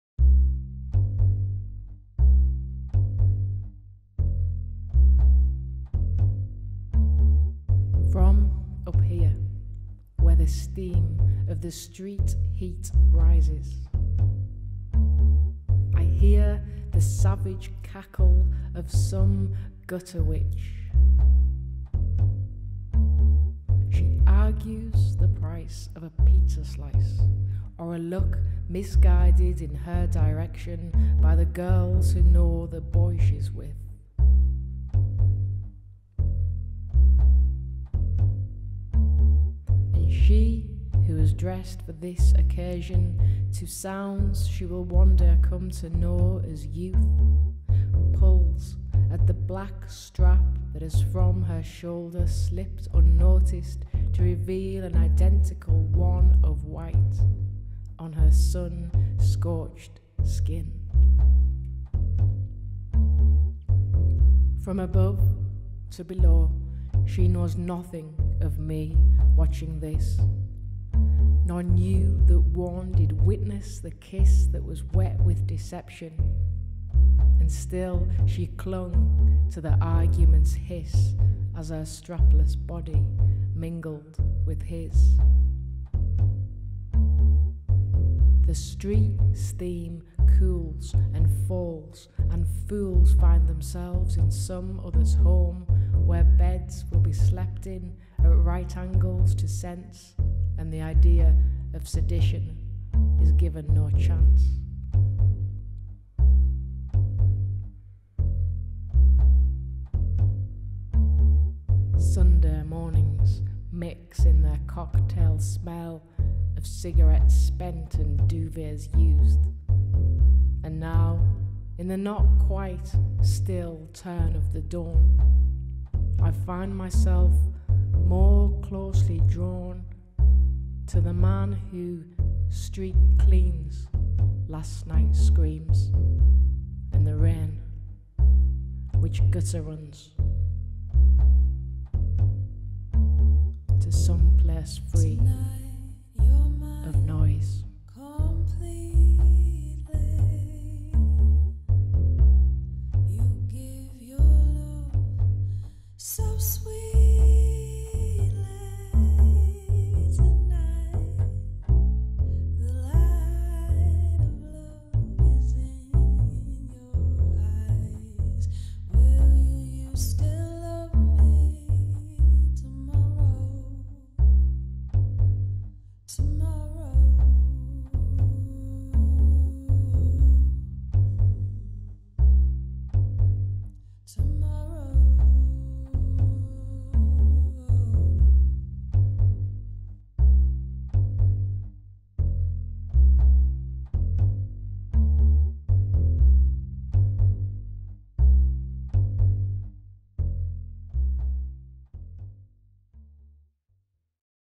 GUTTER WITCH
Video poem inspired by living above a Pizza Shop in Whitehaven. Featuring Sarah Grace Horley.